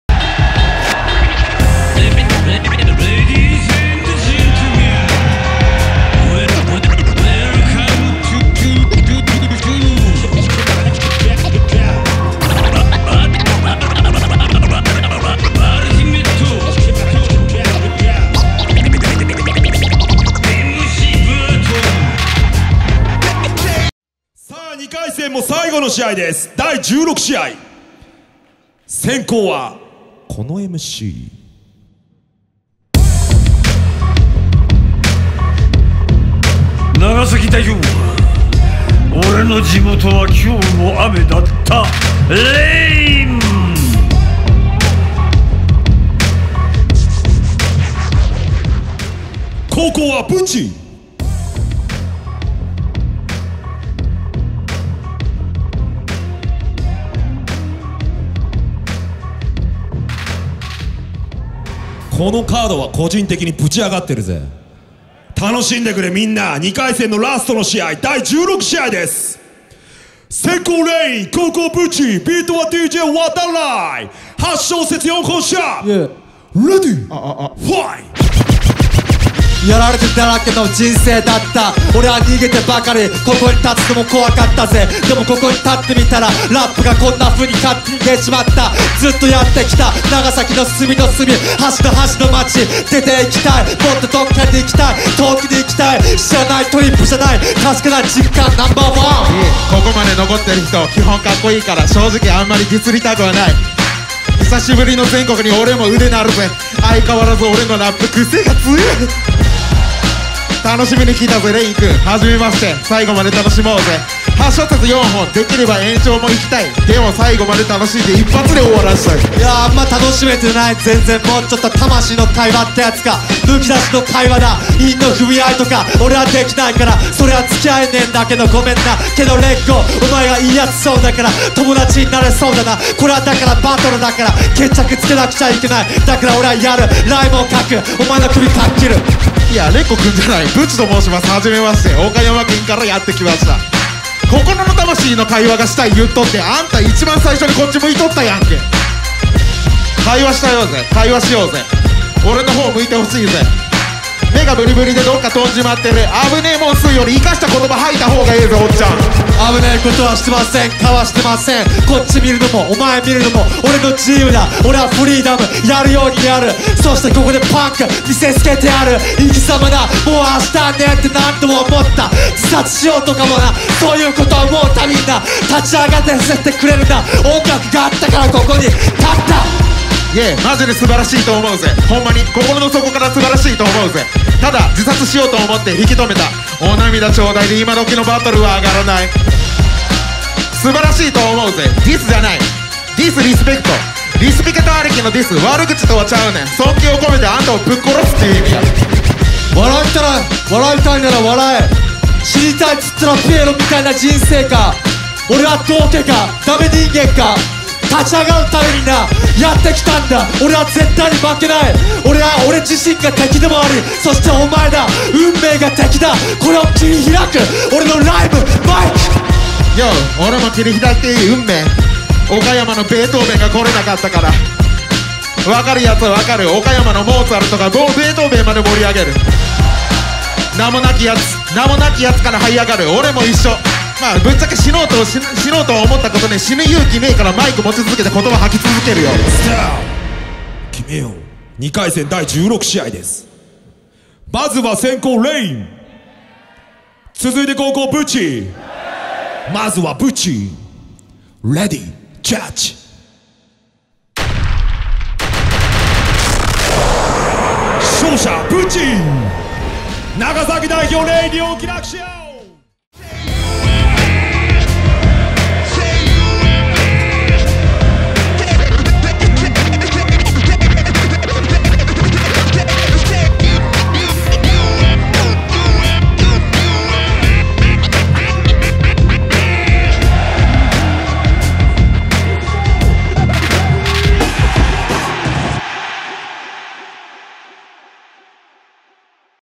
"玲音 vs BUCHI" UMB2019 GRAND CHAMPIONSHIP 2回戦 第16試合
UMB2019 GRAND CHAMPIONSHIP
2回戦 第16試合
"玲音 vs BUCHI"

12/28(SAT)
@新木場STUDIO COAST

紅桜との伝説的とも言えるバトルを準決勝にて繰り広げ、決勝ではAuthorityを気迫で制したMU-TONが、平成最後のUMBチャンピオンとなった『UMB GRAND CHAMPIONSHIP 2018』から早一年。
令和最初のチャンピオンを決めるべく、2019年12月28日に新木場STUDIO COASTにて、通算15回目となる『UMB GRAND CHAMPIONSHIP 2019』が開催された。
　計48名のバトルMCたちが勢ぞろいし、ステージ上にて約５時間に亘って繰り広げられた数々の名勝負。UMBではお馴染みの常連組から、UMB本戦への進出は初というニューカマーまで、年齢層やスタイルも含めて様々なメンツが入り乱れ、１回戦目から番狂わせとも言えるような波乱のバトルが展開していく。